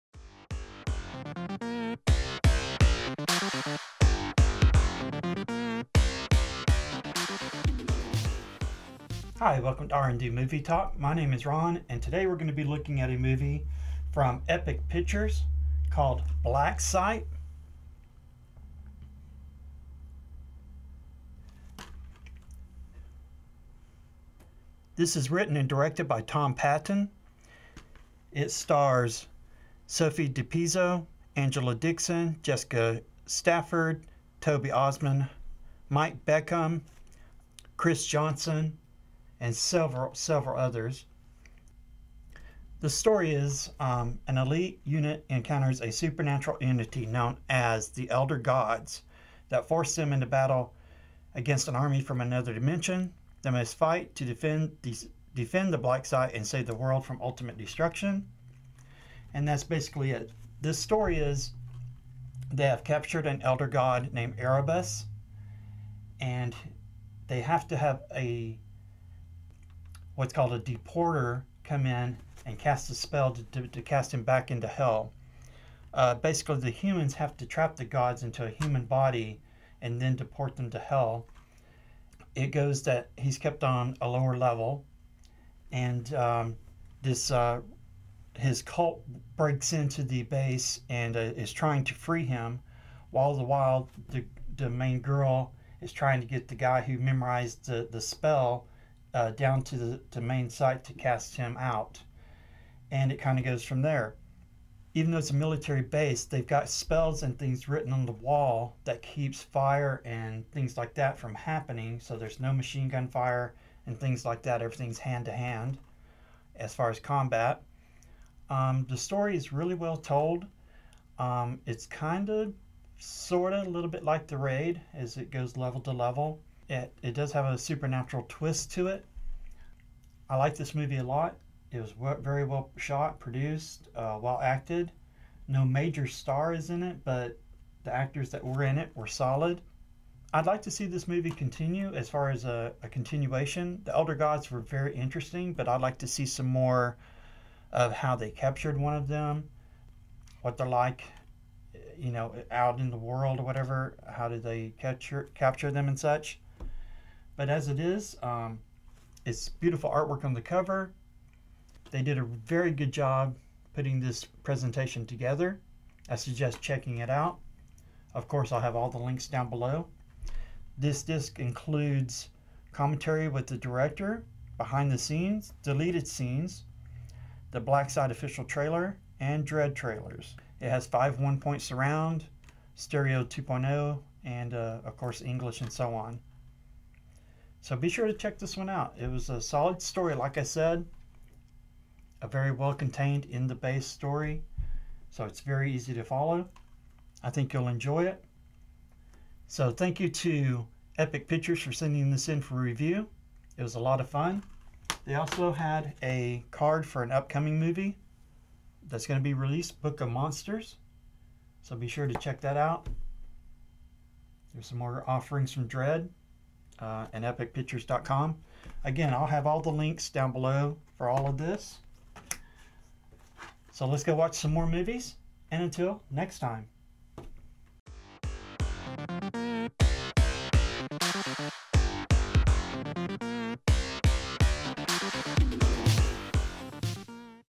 BLACK SITE Review - Epic Pictures (2019)
"An elite military unit encounters a supernatural entity, known as The Elder Gods, that forces them into battle against an army from another dimension. They must fight together to defend the Black Site and save the world from ultimate destruction."

BLACK SITE is a horror action film directed by Tom Paton and is from Epic Pictures (2019).This film is in stores now, or you can find where to purchase BLACK SITE in the links below.